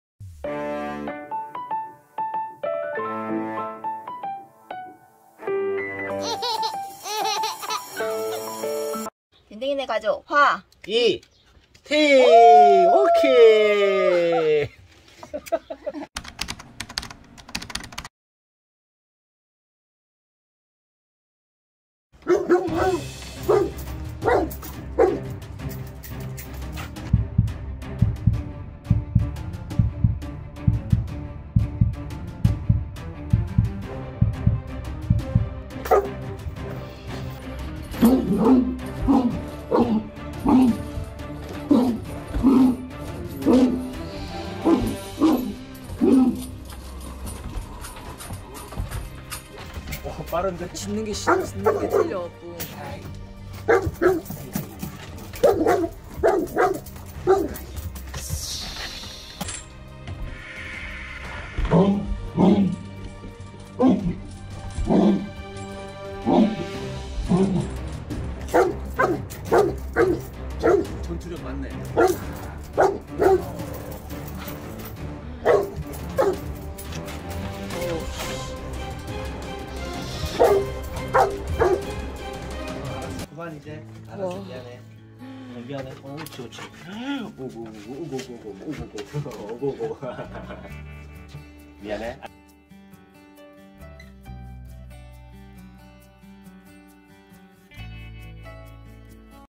방어만랩 !! 날카로움 /대형견/카네코르소/ cane corso/뿡뿡피지컬
위협이 가해지면 방어만랩
덴댕이 ~ 순둥순둥 하지만 가족과 재산을 지키고 위험으로 부터
우리를 지캬주는덴~